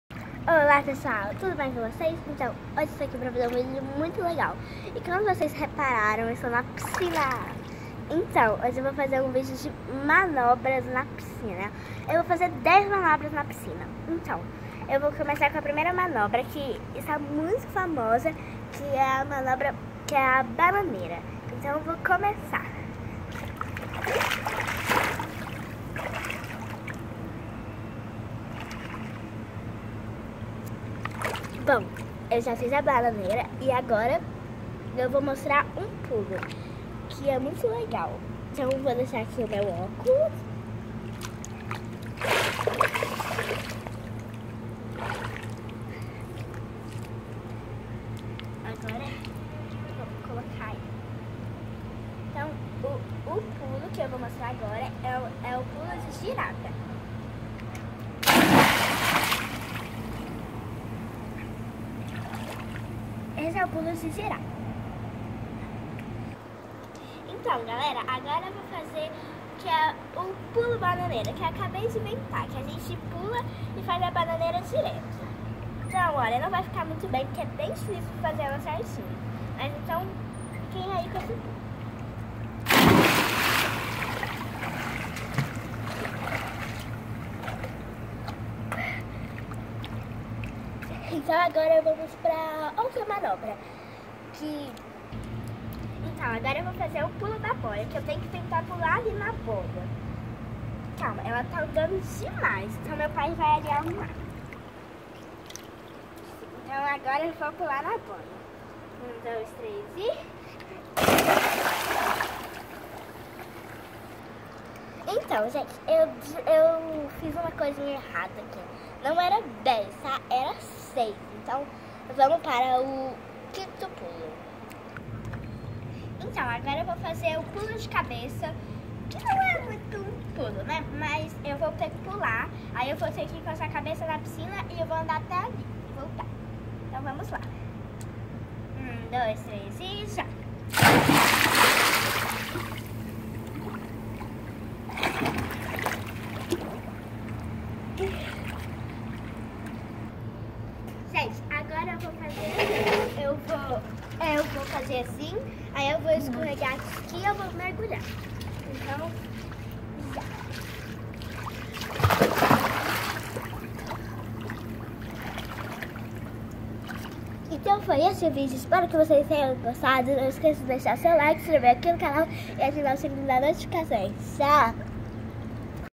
Manobras na piscina.
Seis saltos na piscina do condomínio.